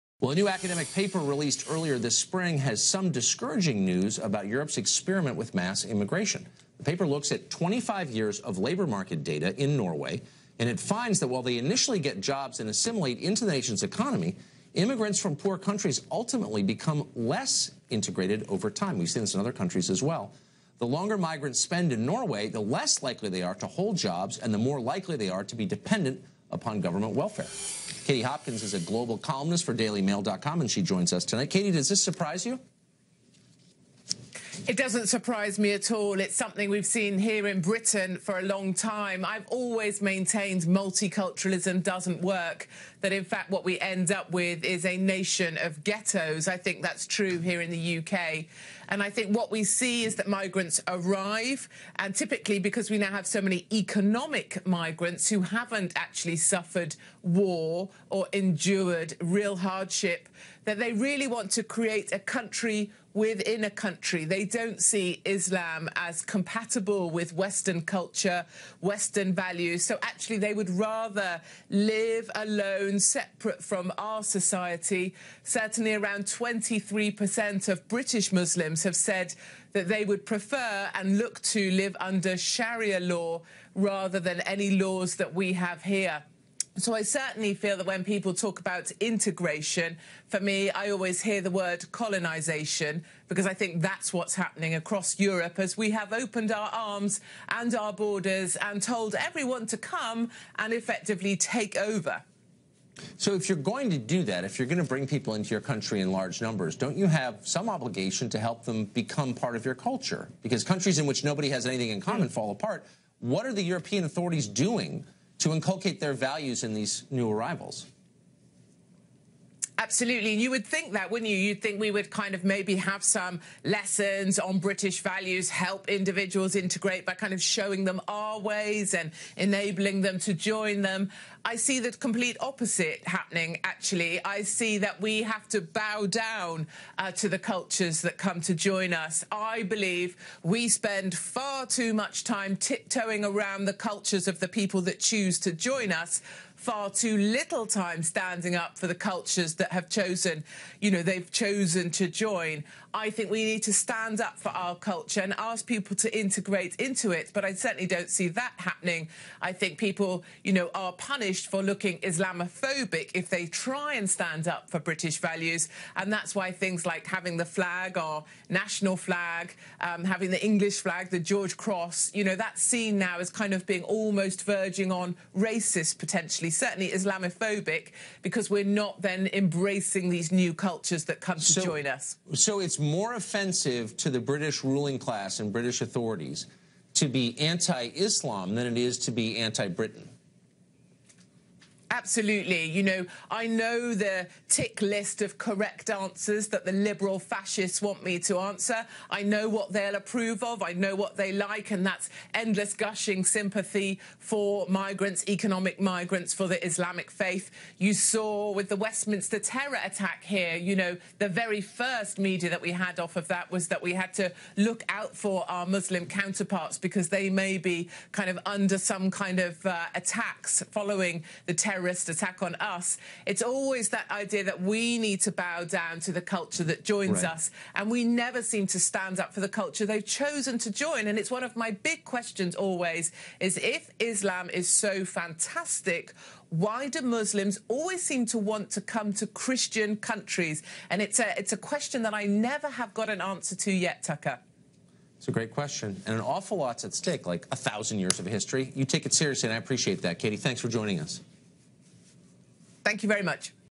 Multiculturalism DOESN'T Work! Tucker and Katie Hopkins Discuss Europe's Migrant Crisis
Tucker interviews British columnist Katie Hopkins about Europe's Migrant Crisis.